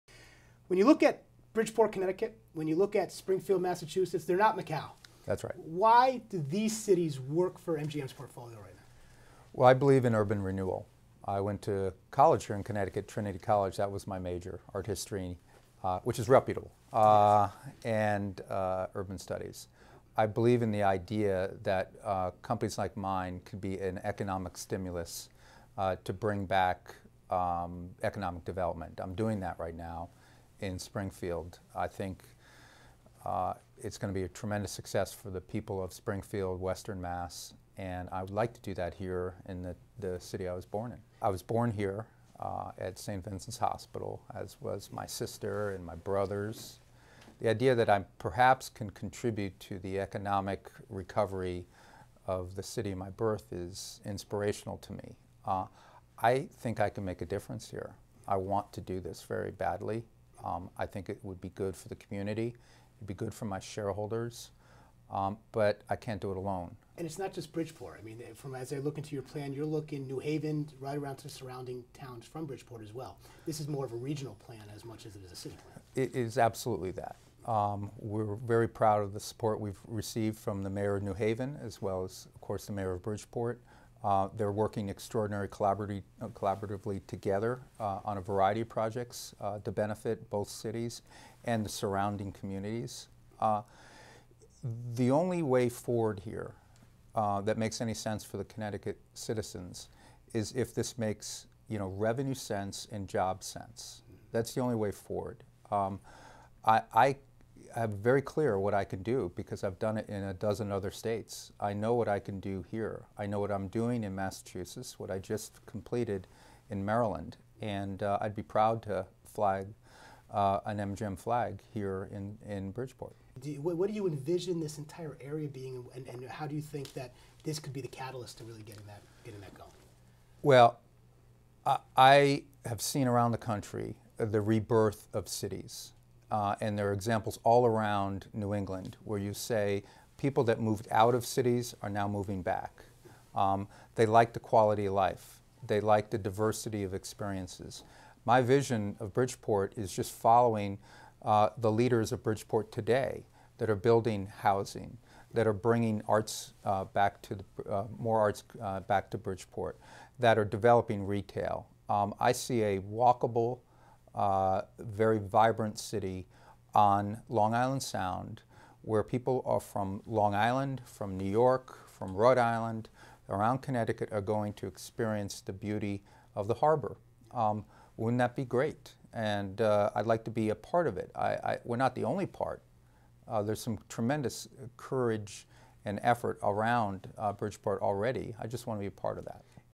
Web Extra: Jim Murren on why a casino would work in Bridgeport
The MGM Casinos CEO talks about what contributing to the economy in his birthplace city would mean to him.